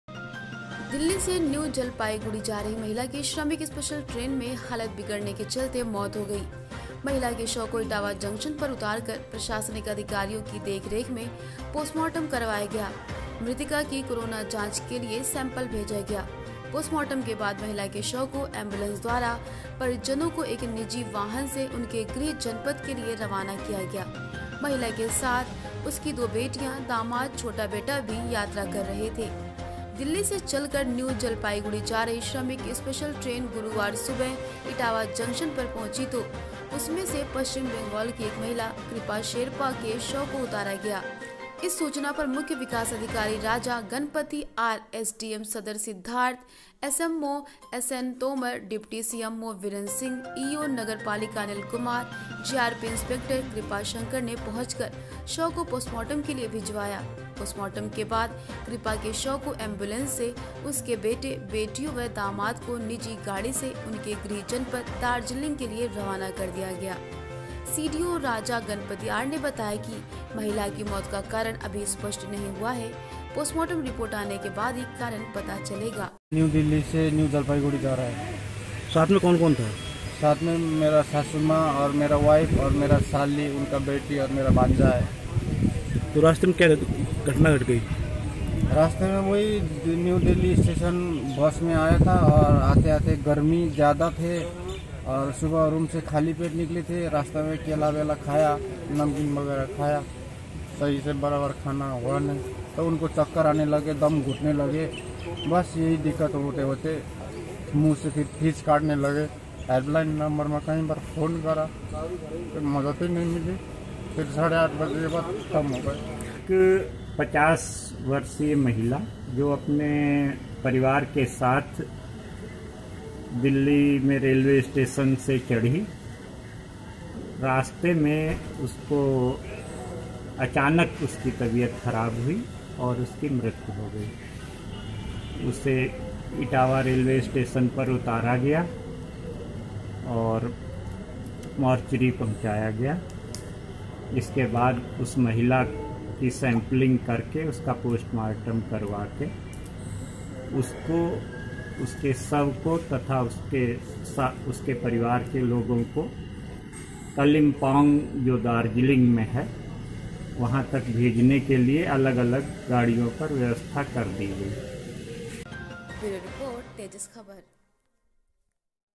ETAWAH: भारी हुआ सफर, श्रमिक स्पेशल ट्रेन में महिला की मौत, जा रही थी पश्चिम बंगाल...
दिल्ली से बंगाल जा रही महिला की श्रमिक स्पेशल ट्रेन में मौत
इटावा। गुरुवार को
दिल्ली से न्यू जलपाईगुड़ी जा रही महिला की श्रमिक स्पेशल ट्रेन में हालत बिगड़ने के चलते मौत हो गई। महिला के शव को इटावा जंक्शन पर उतार कर  प्रशासनिक अधिकारियों की देखरेख में पोस्टमाॅर्टम करवाया गया। मृतका की कोरोना जांच के लिए भी सैंपल भेजा गया है। पोस्टमाॅर्टम के बाद महिला के शव को एंबुलेंस द्वारा तथा परिजनों को एक निजी वाहन से उनके गृह जनपद को रवाना किया गया। महिला के साथ उसकी दो बेटियां, दामाद व छोटा बेटा भी यात्रा कर रहे थे।
दिल्ली से चलकर न्यू जलपाईगुड़ी जा रही श्रमिक स्पेशल ट्रेन गुरुवार सुबह इटावा जंक्शन पर पहुंची तो उसमें से पश्चिम बंगाल की एक महिला कृपा शेरपा के शव को उतारा गया। इस सूचना पर मुख्य विकास अधिकारी राजा गणपति आर, एसडीएम सदर सिद्धार्थ, सीएमओ एनएस तोमर, डिप्टी सीएमओ वीरेन्द्र सिंह, ईओ नगर पालिका अनिल कुमार, जीआरपी इंस्पेक्टर कृपाल शंकर आदि ने पहुंच कर शव को पोस्टमाॅर्टम के लिए पहुंचाया। पोस्टमाॅर्टम के बाद कृपा के शव को एंबुलेंस से तथा उसके बेटे, बेटियों व दामाद को निजी गाड़ी से उनके गृह जनपद दार्जिलिंग को रवाना किया गया। सीडीओ राजा गणपति आर ने बताया कि महिला की मौत का कारण अभी स्पष्ट नहीं हुआ है, पोस्टमाॅर्टम रिपोर्ट आने के बाद ही मौत का कारण पता चल सकेगा। उन्होंने बताया कि महिला की कोरोना जांच के लिए भी सैंपल भेजा गया है।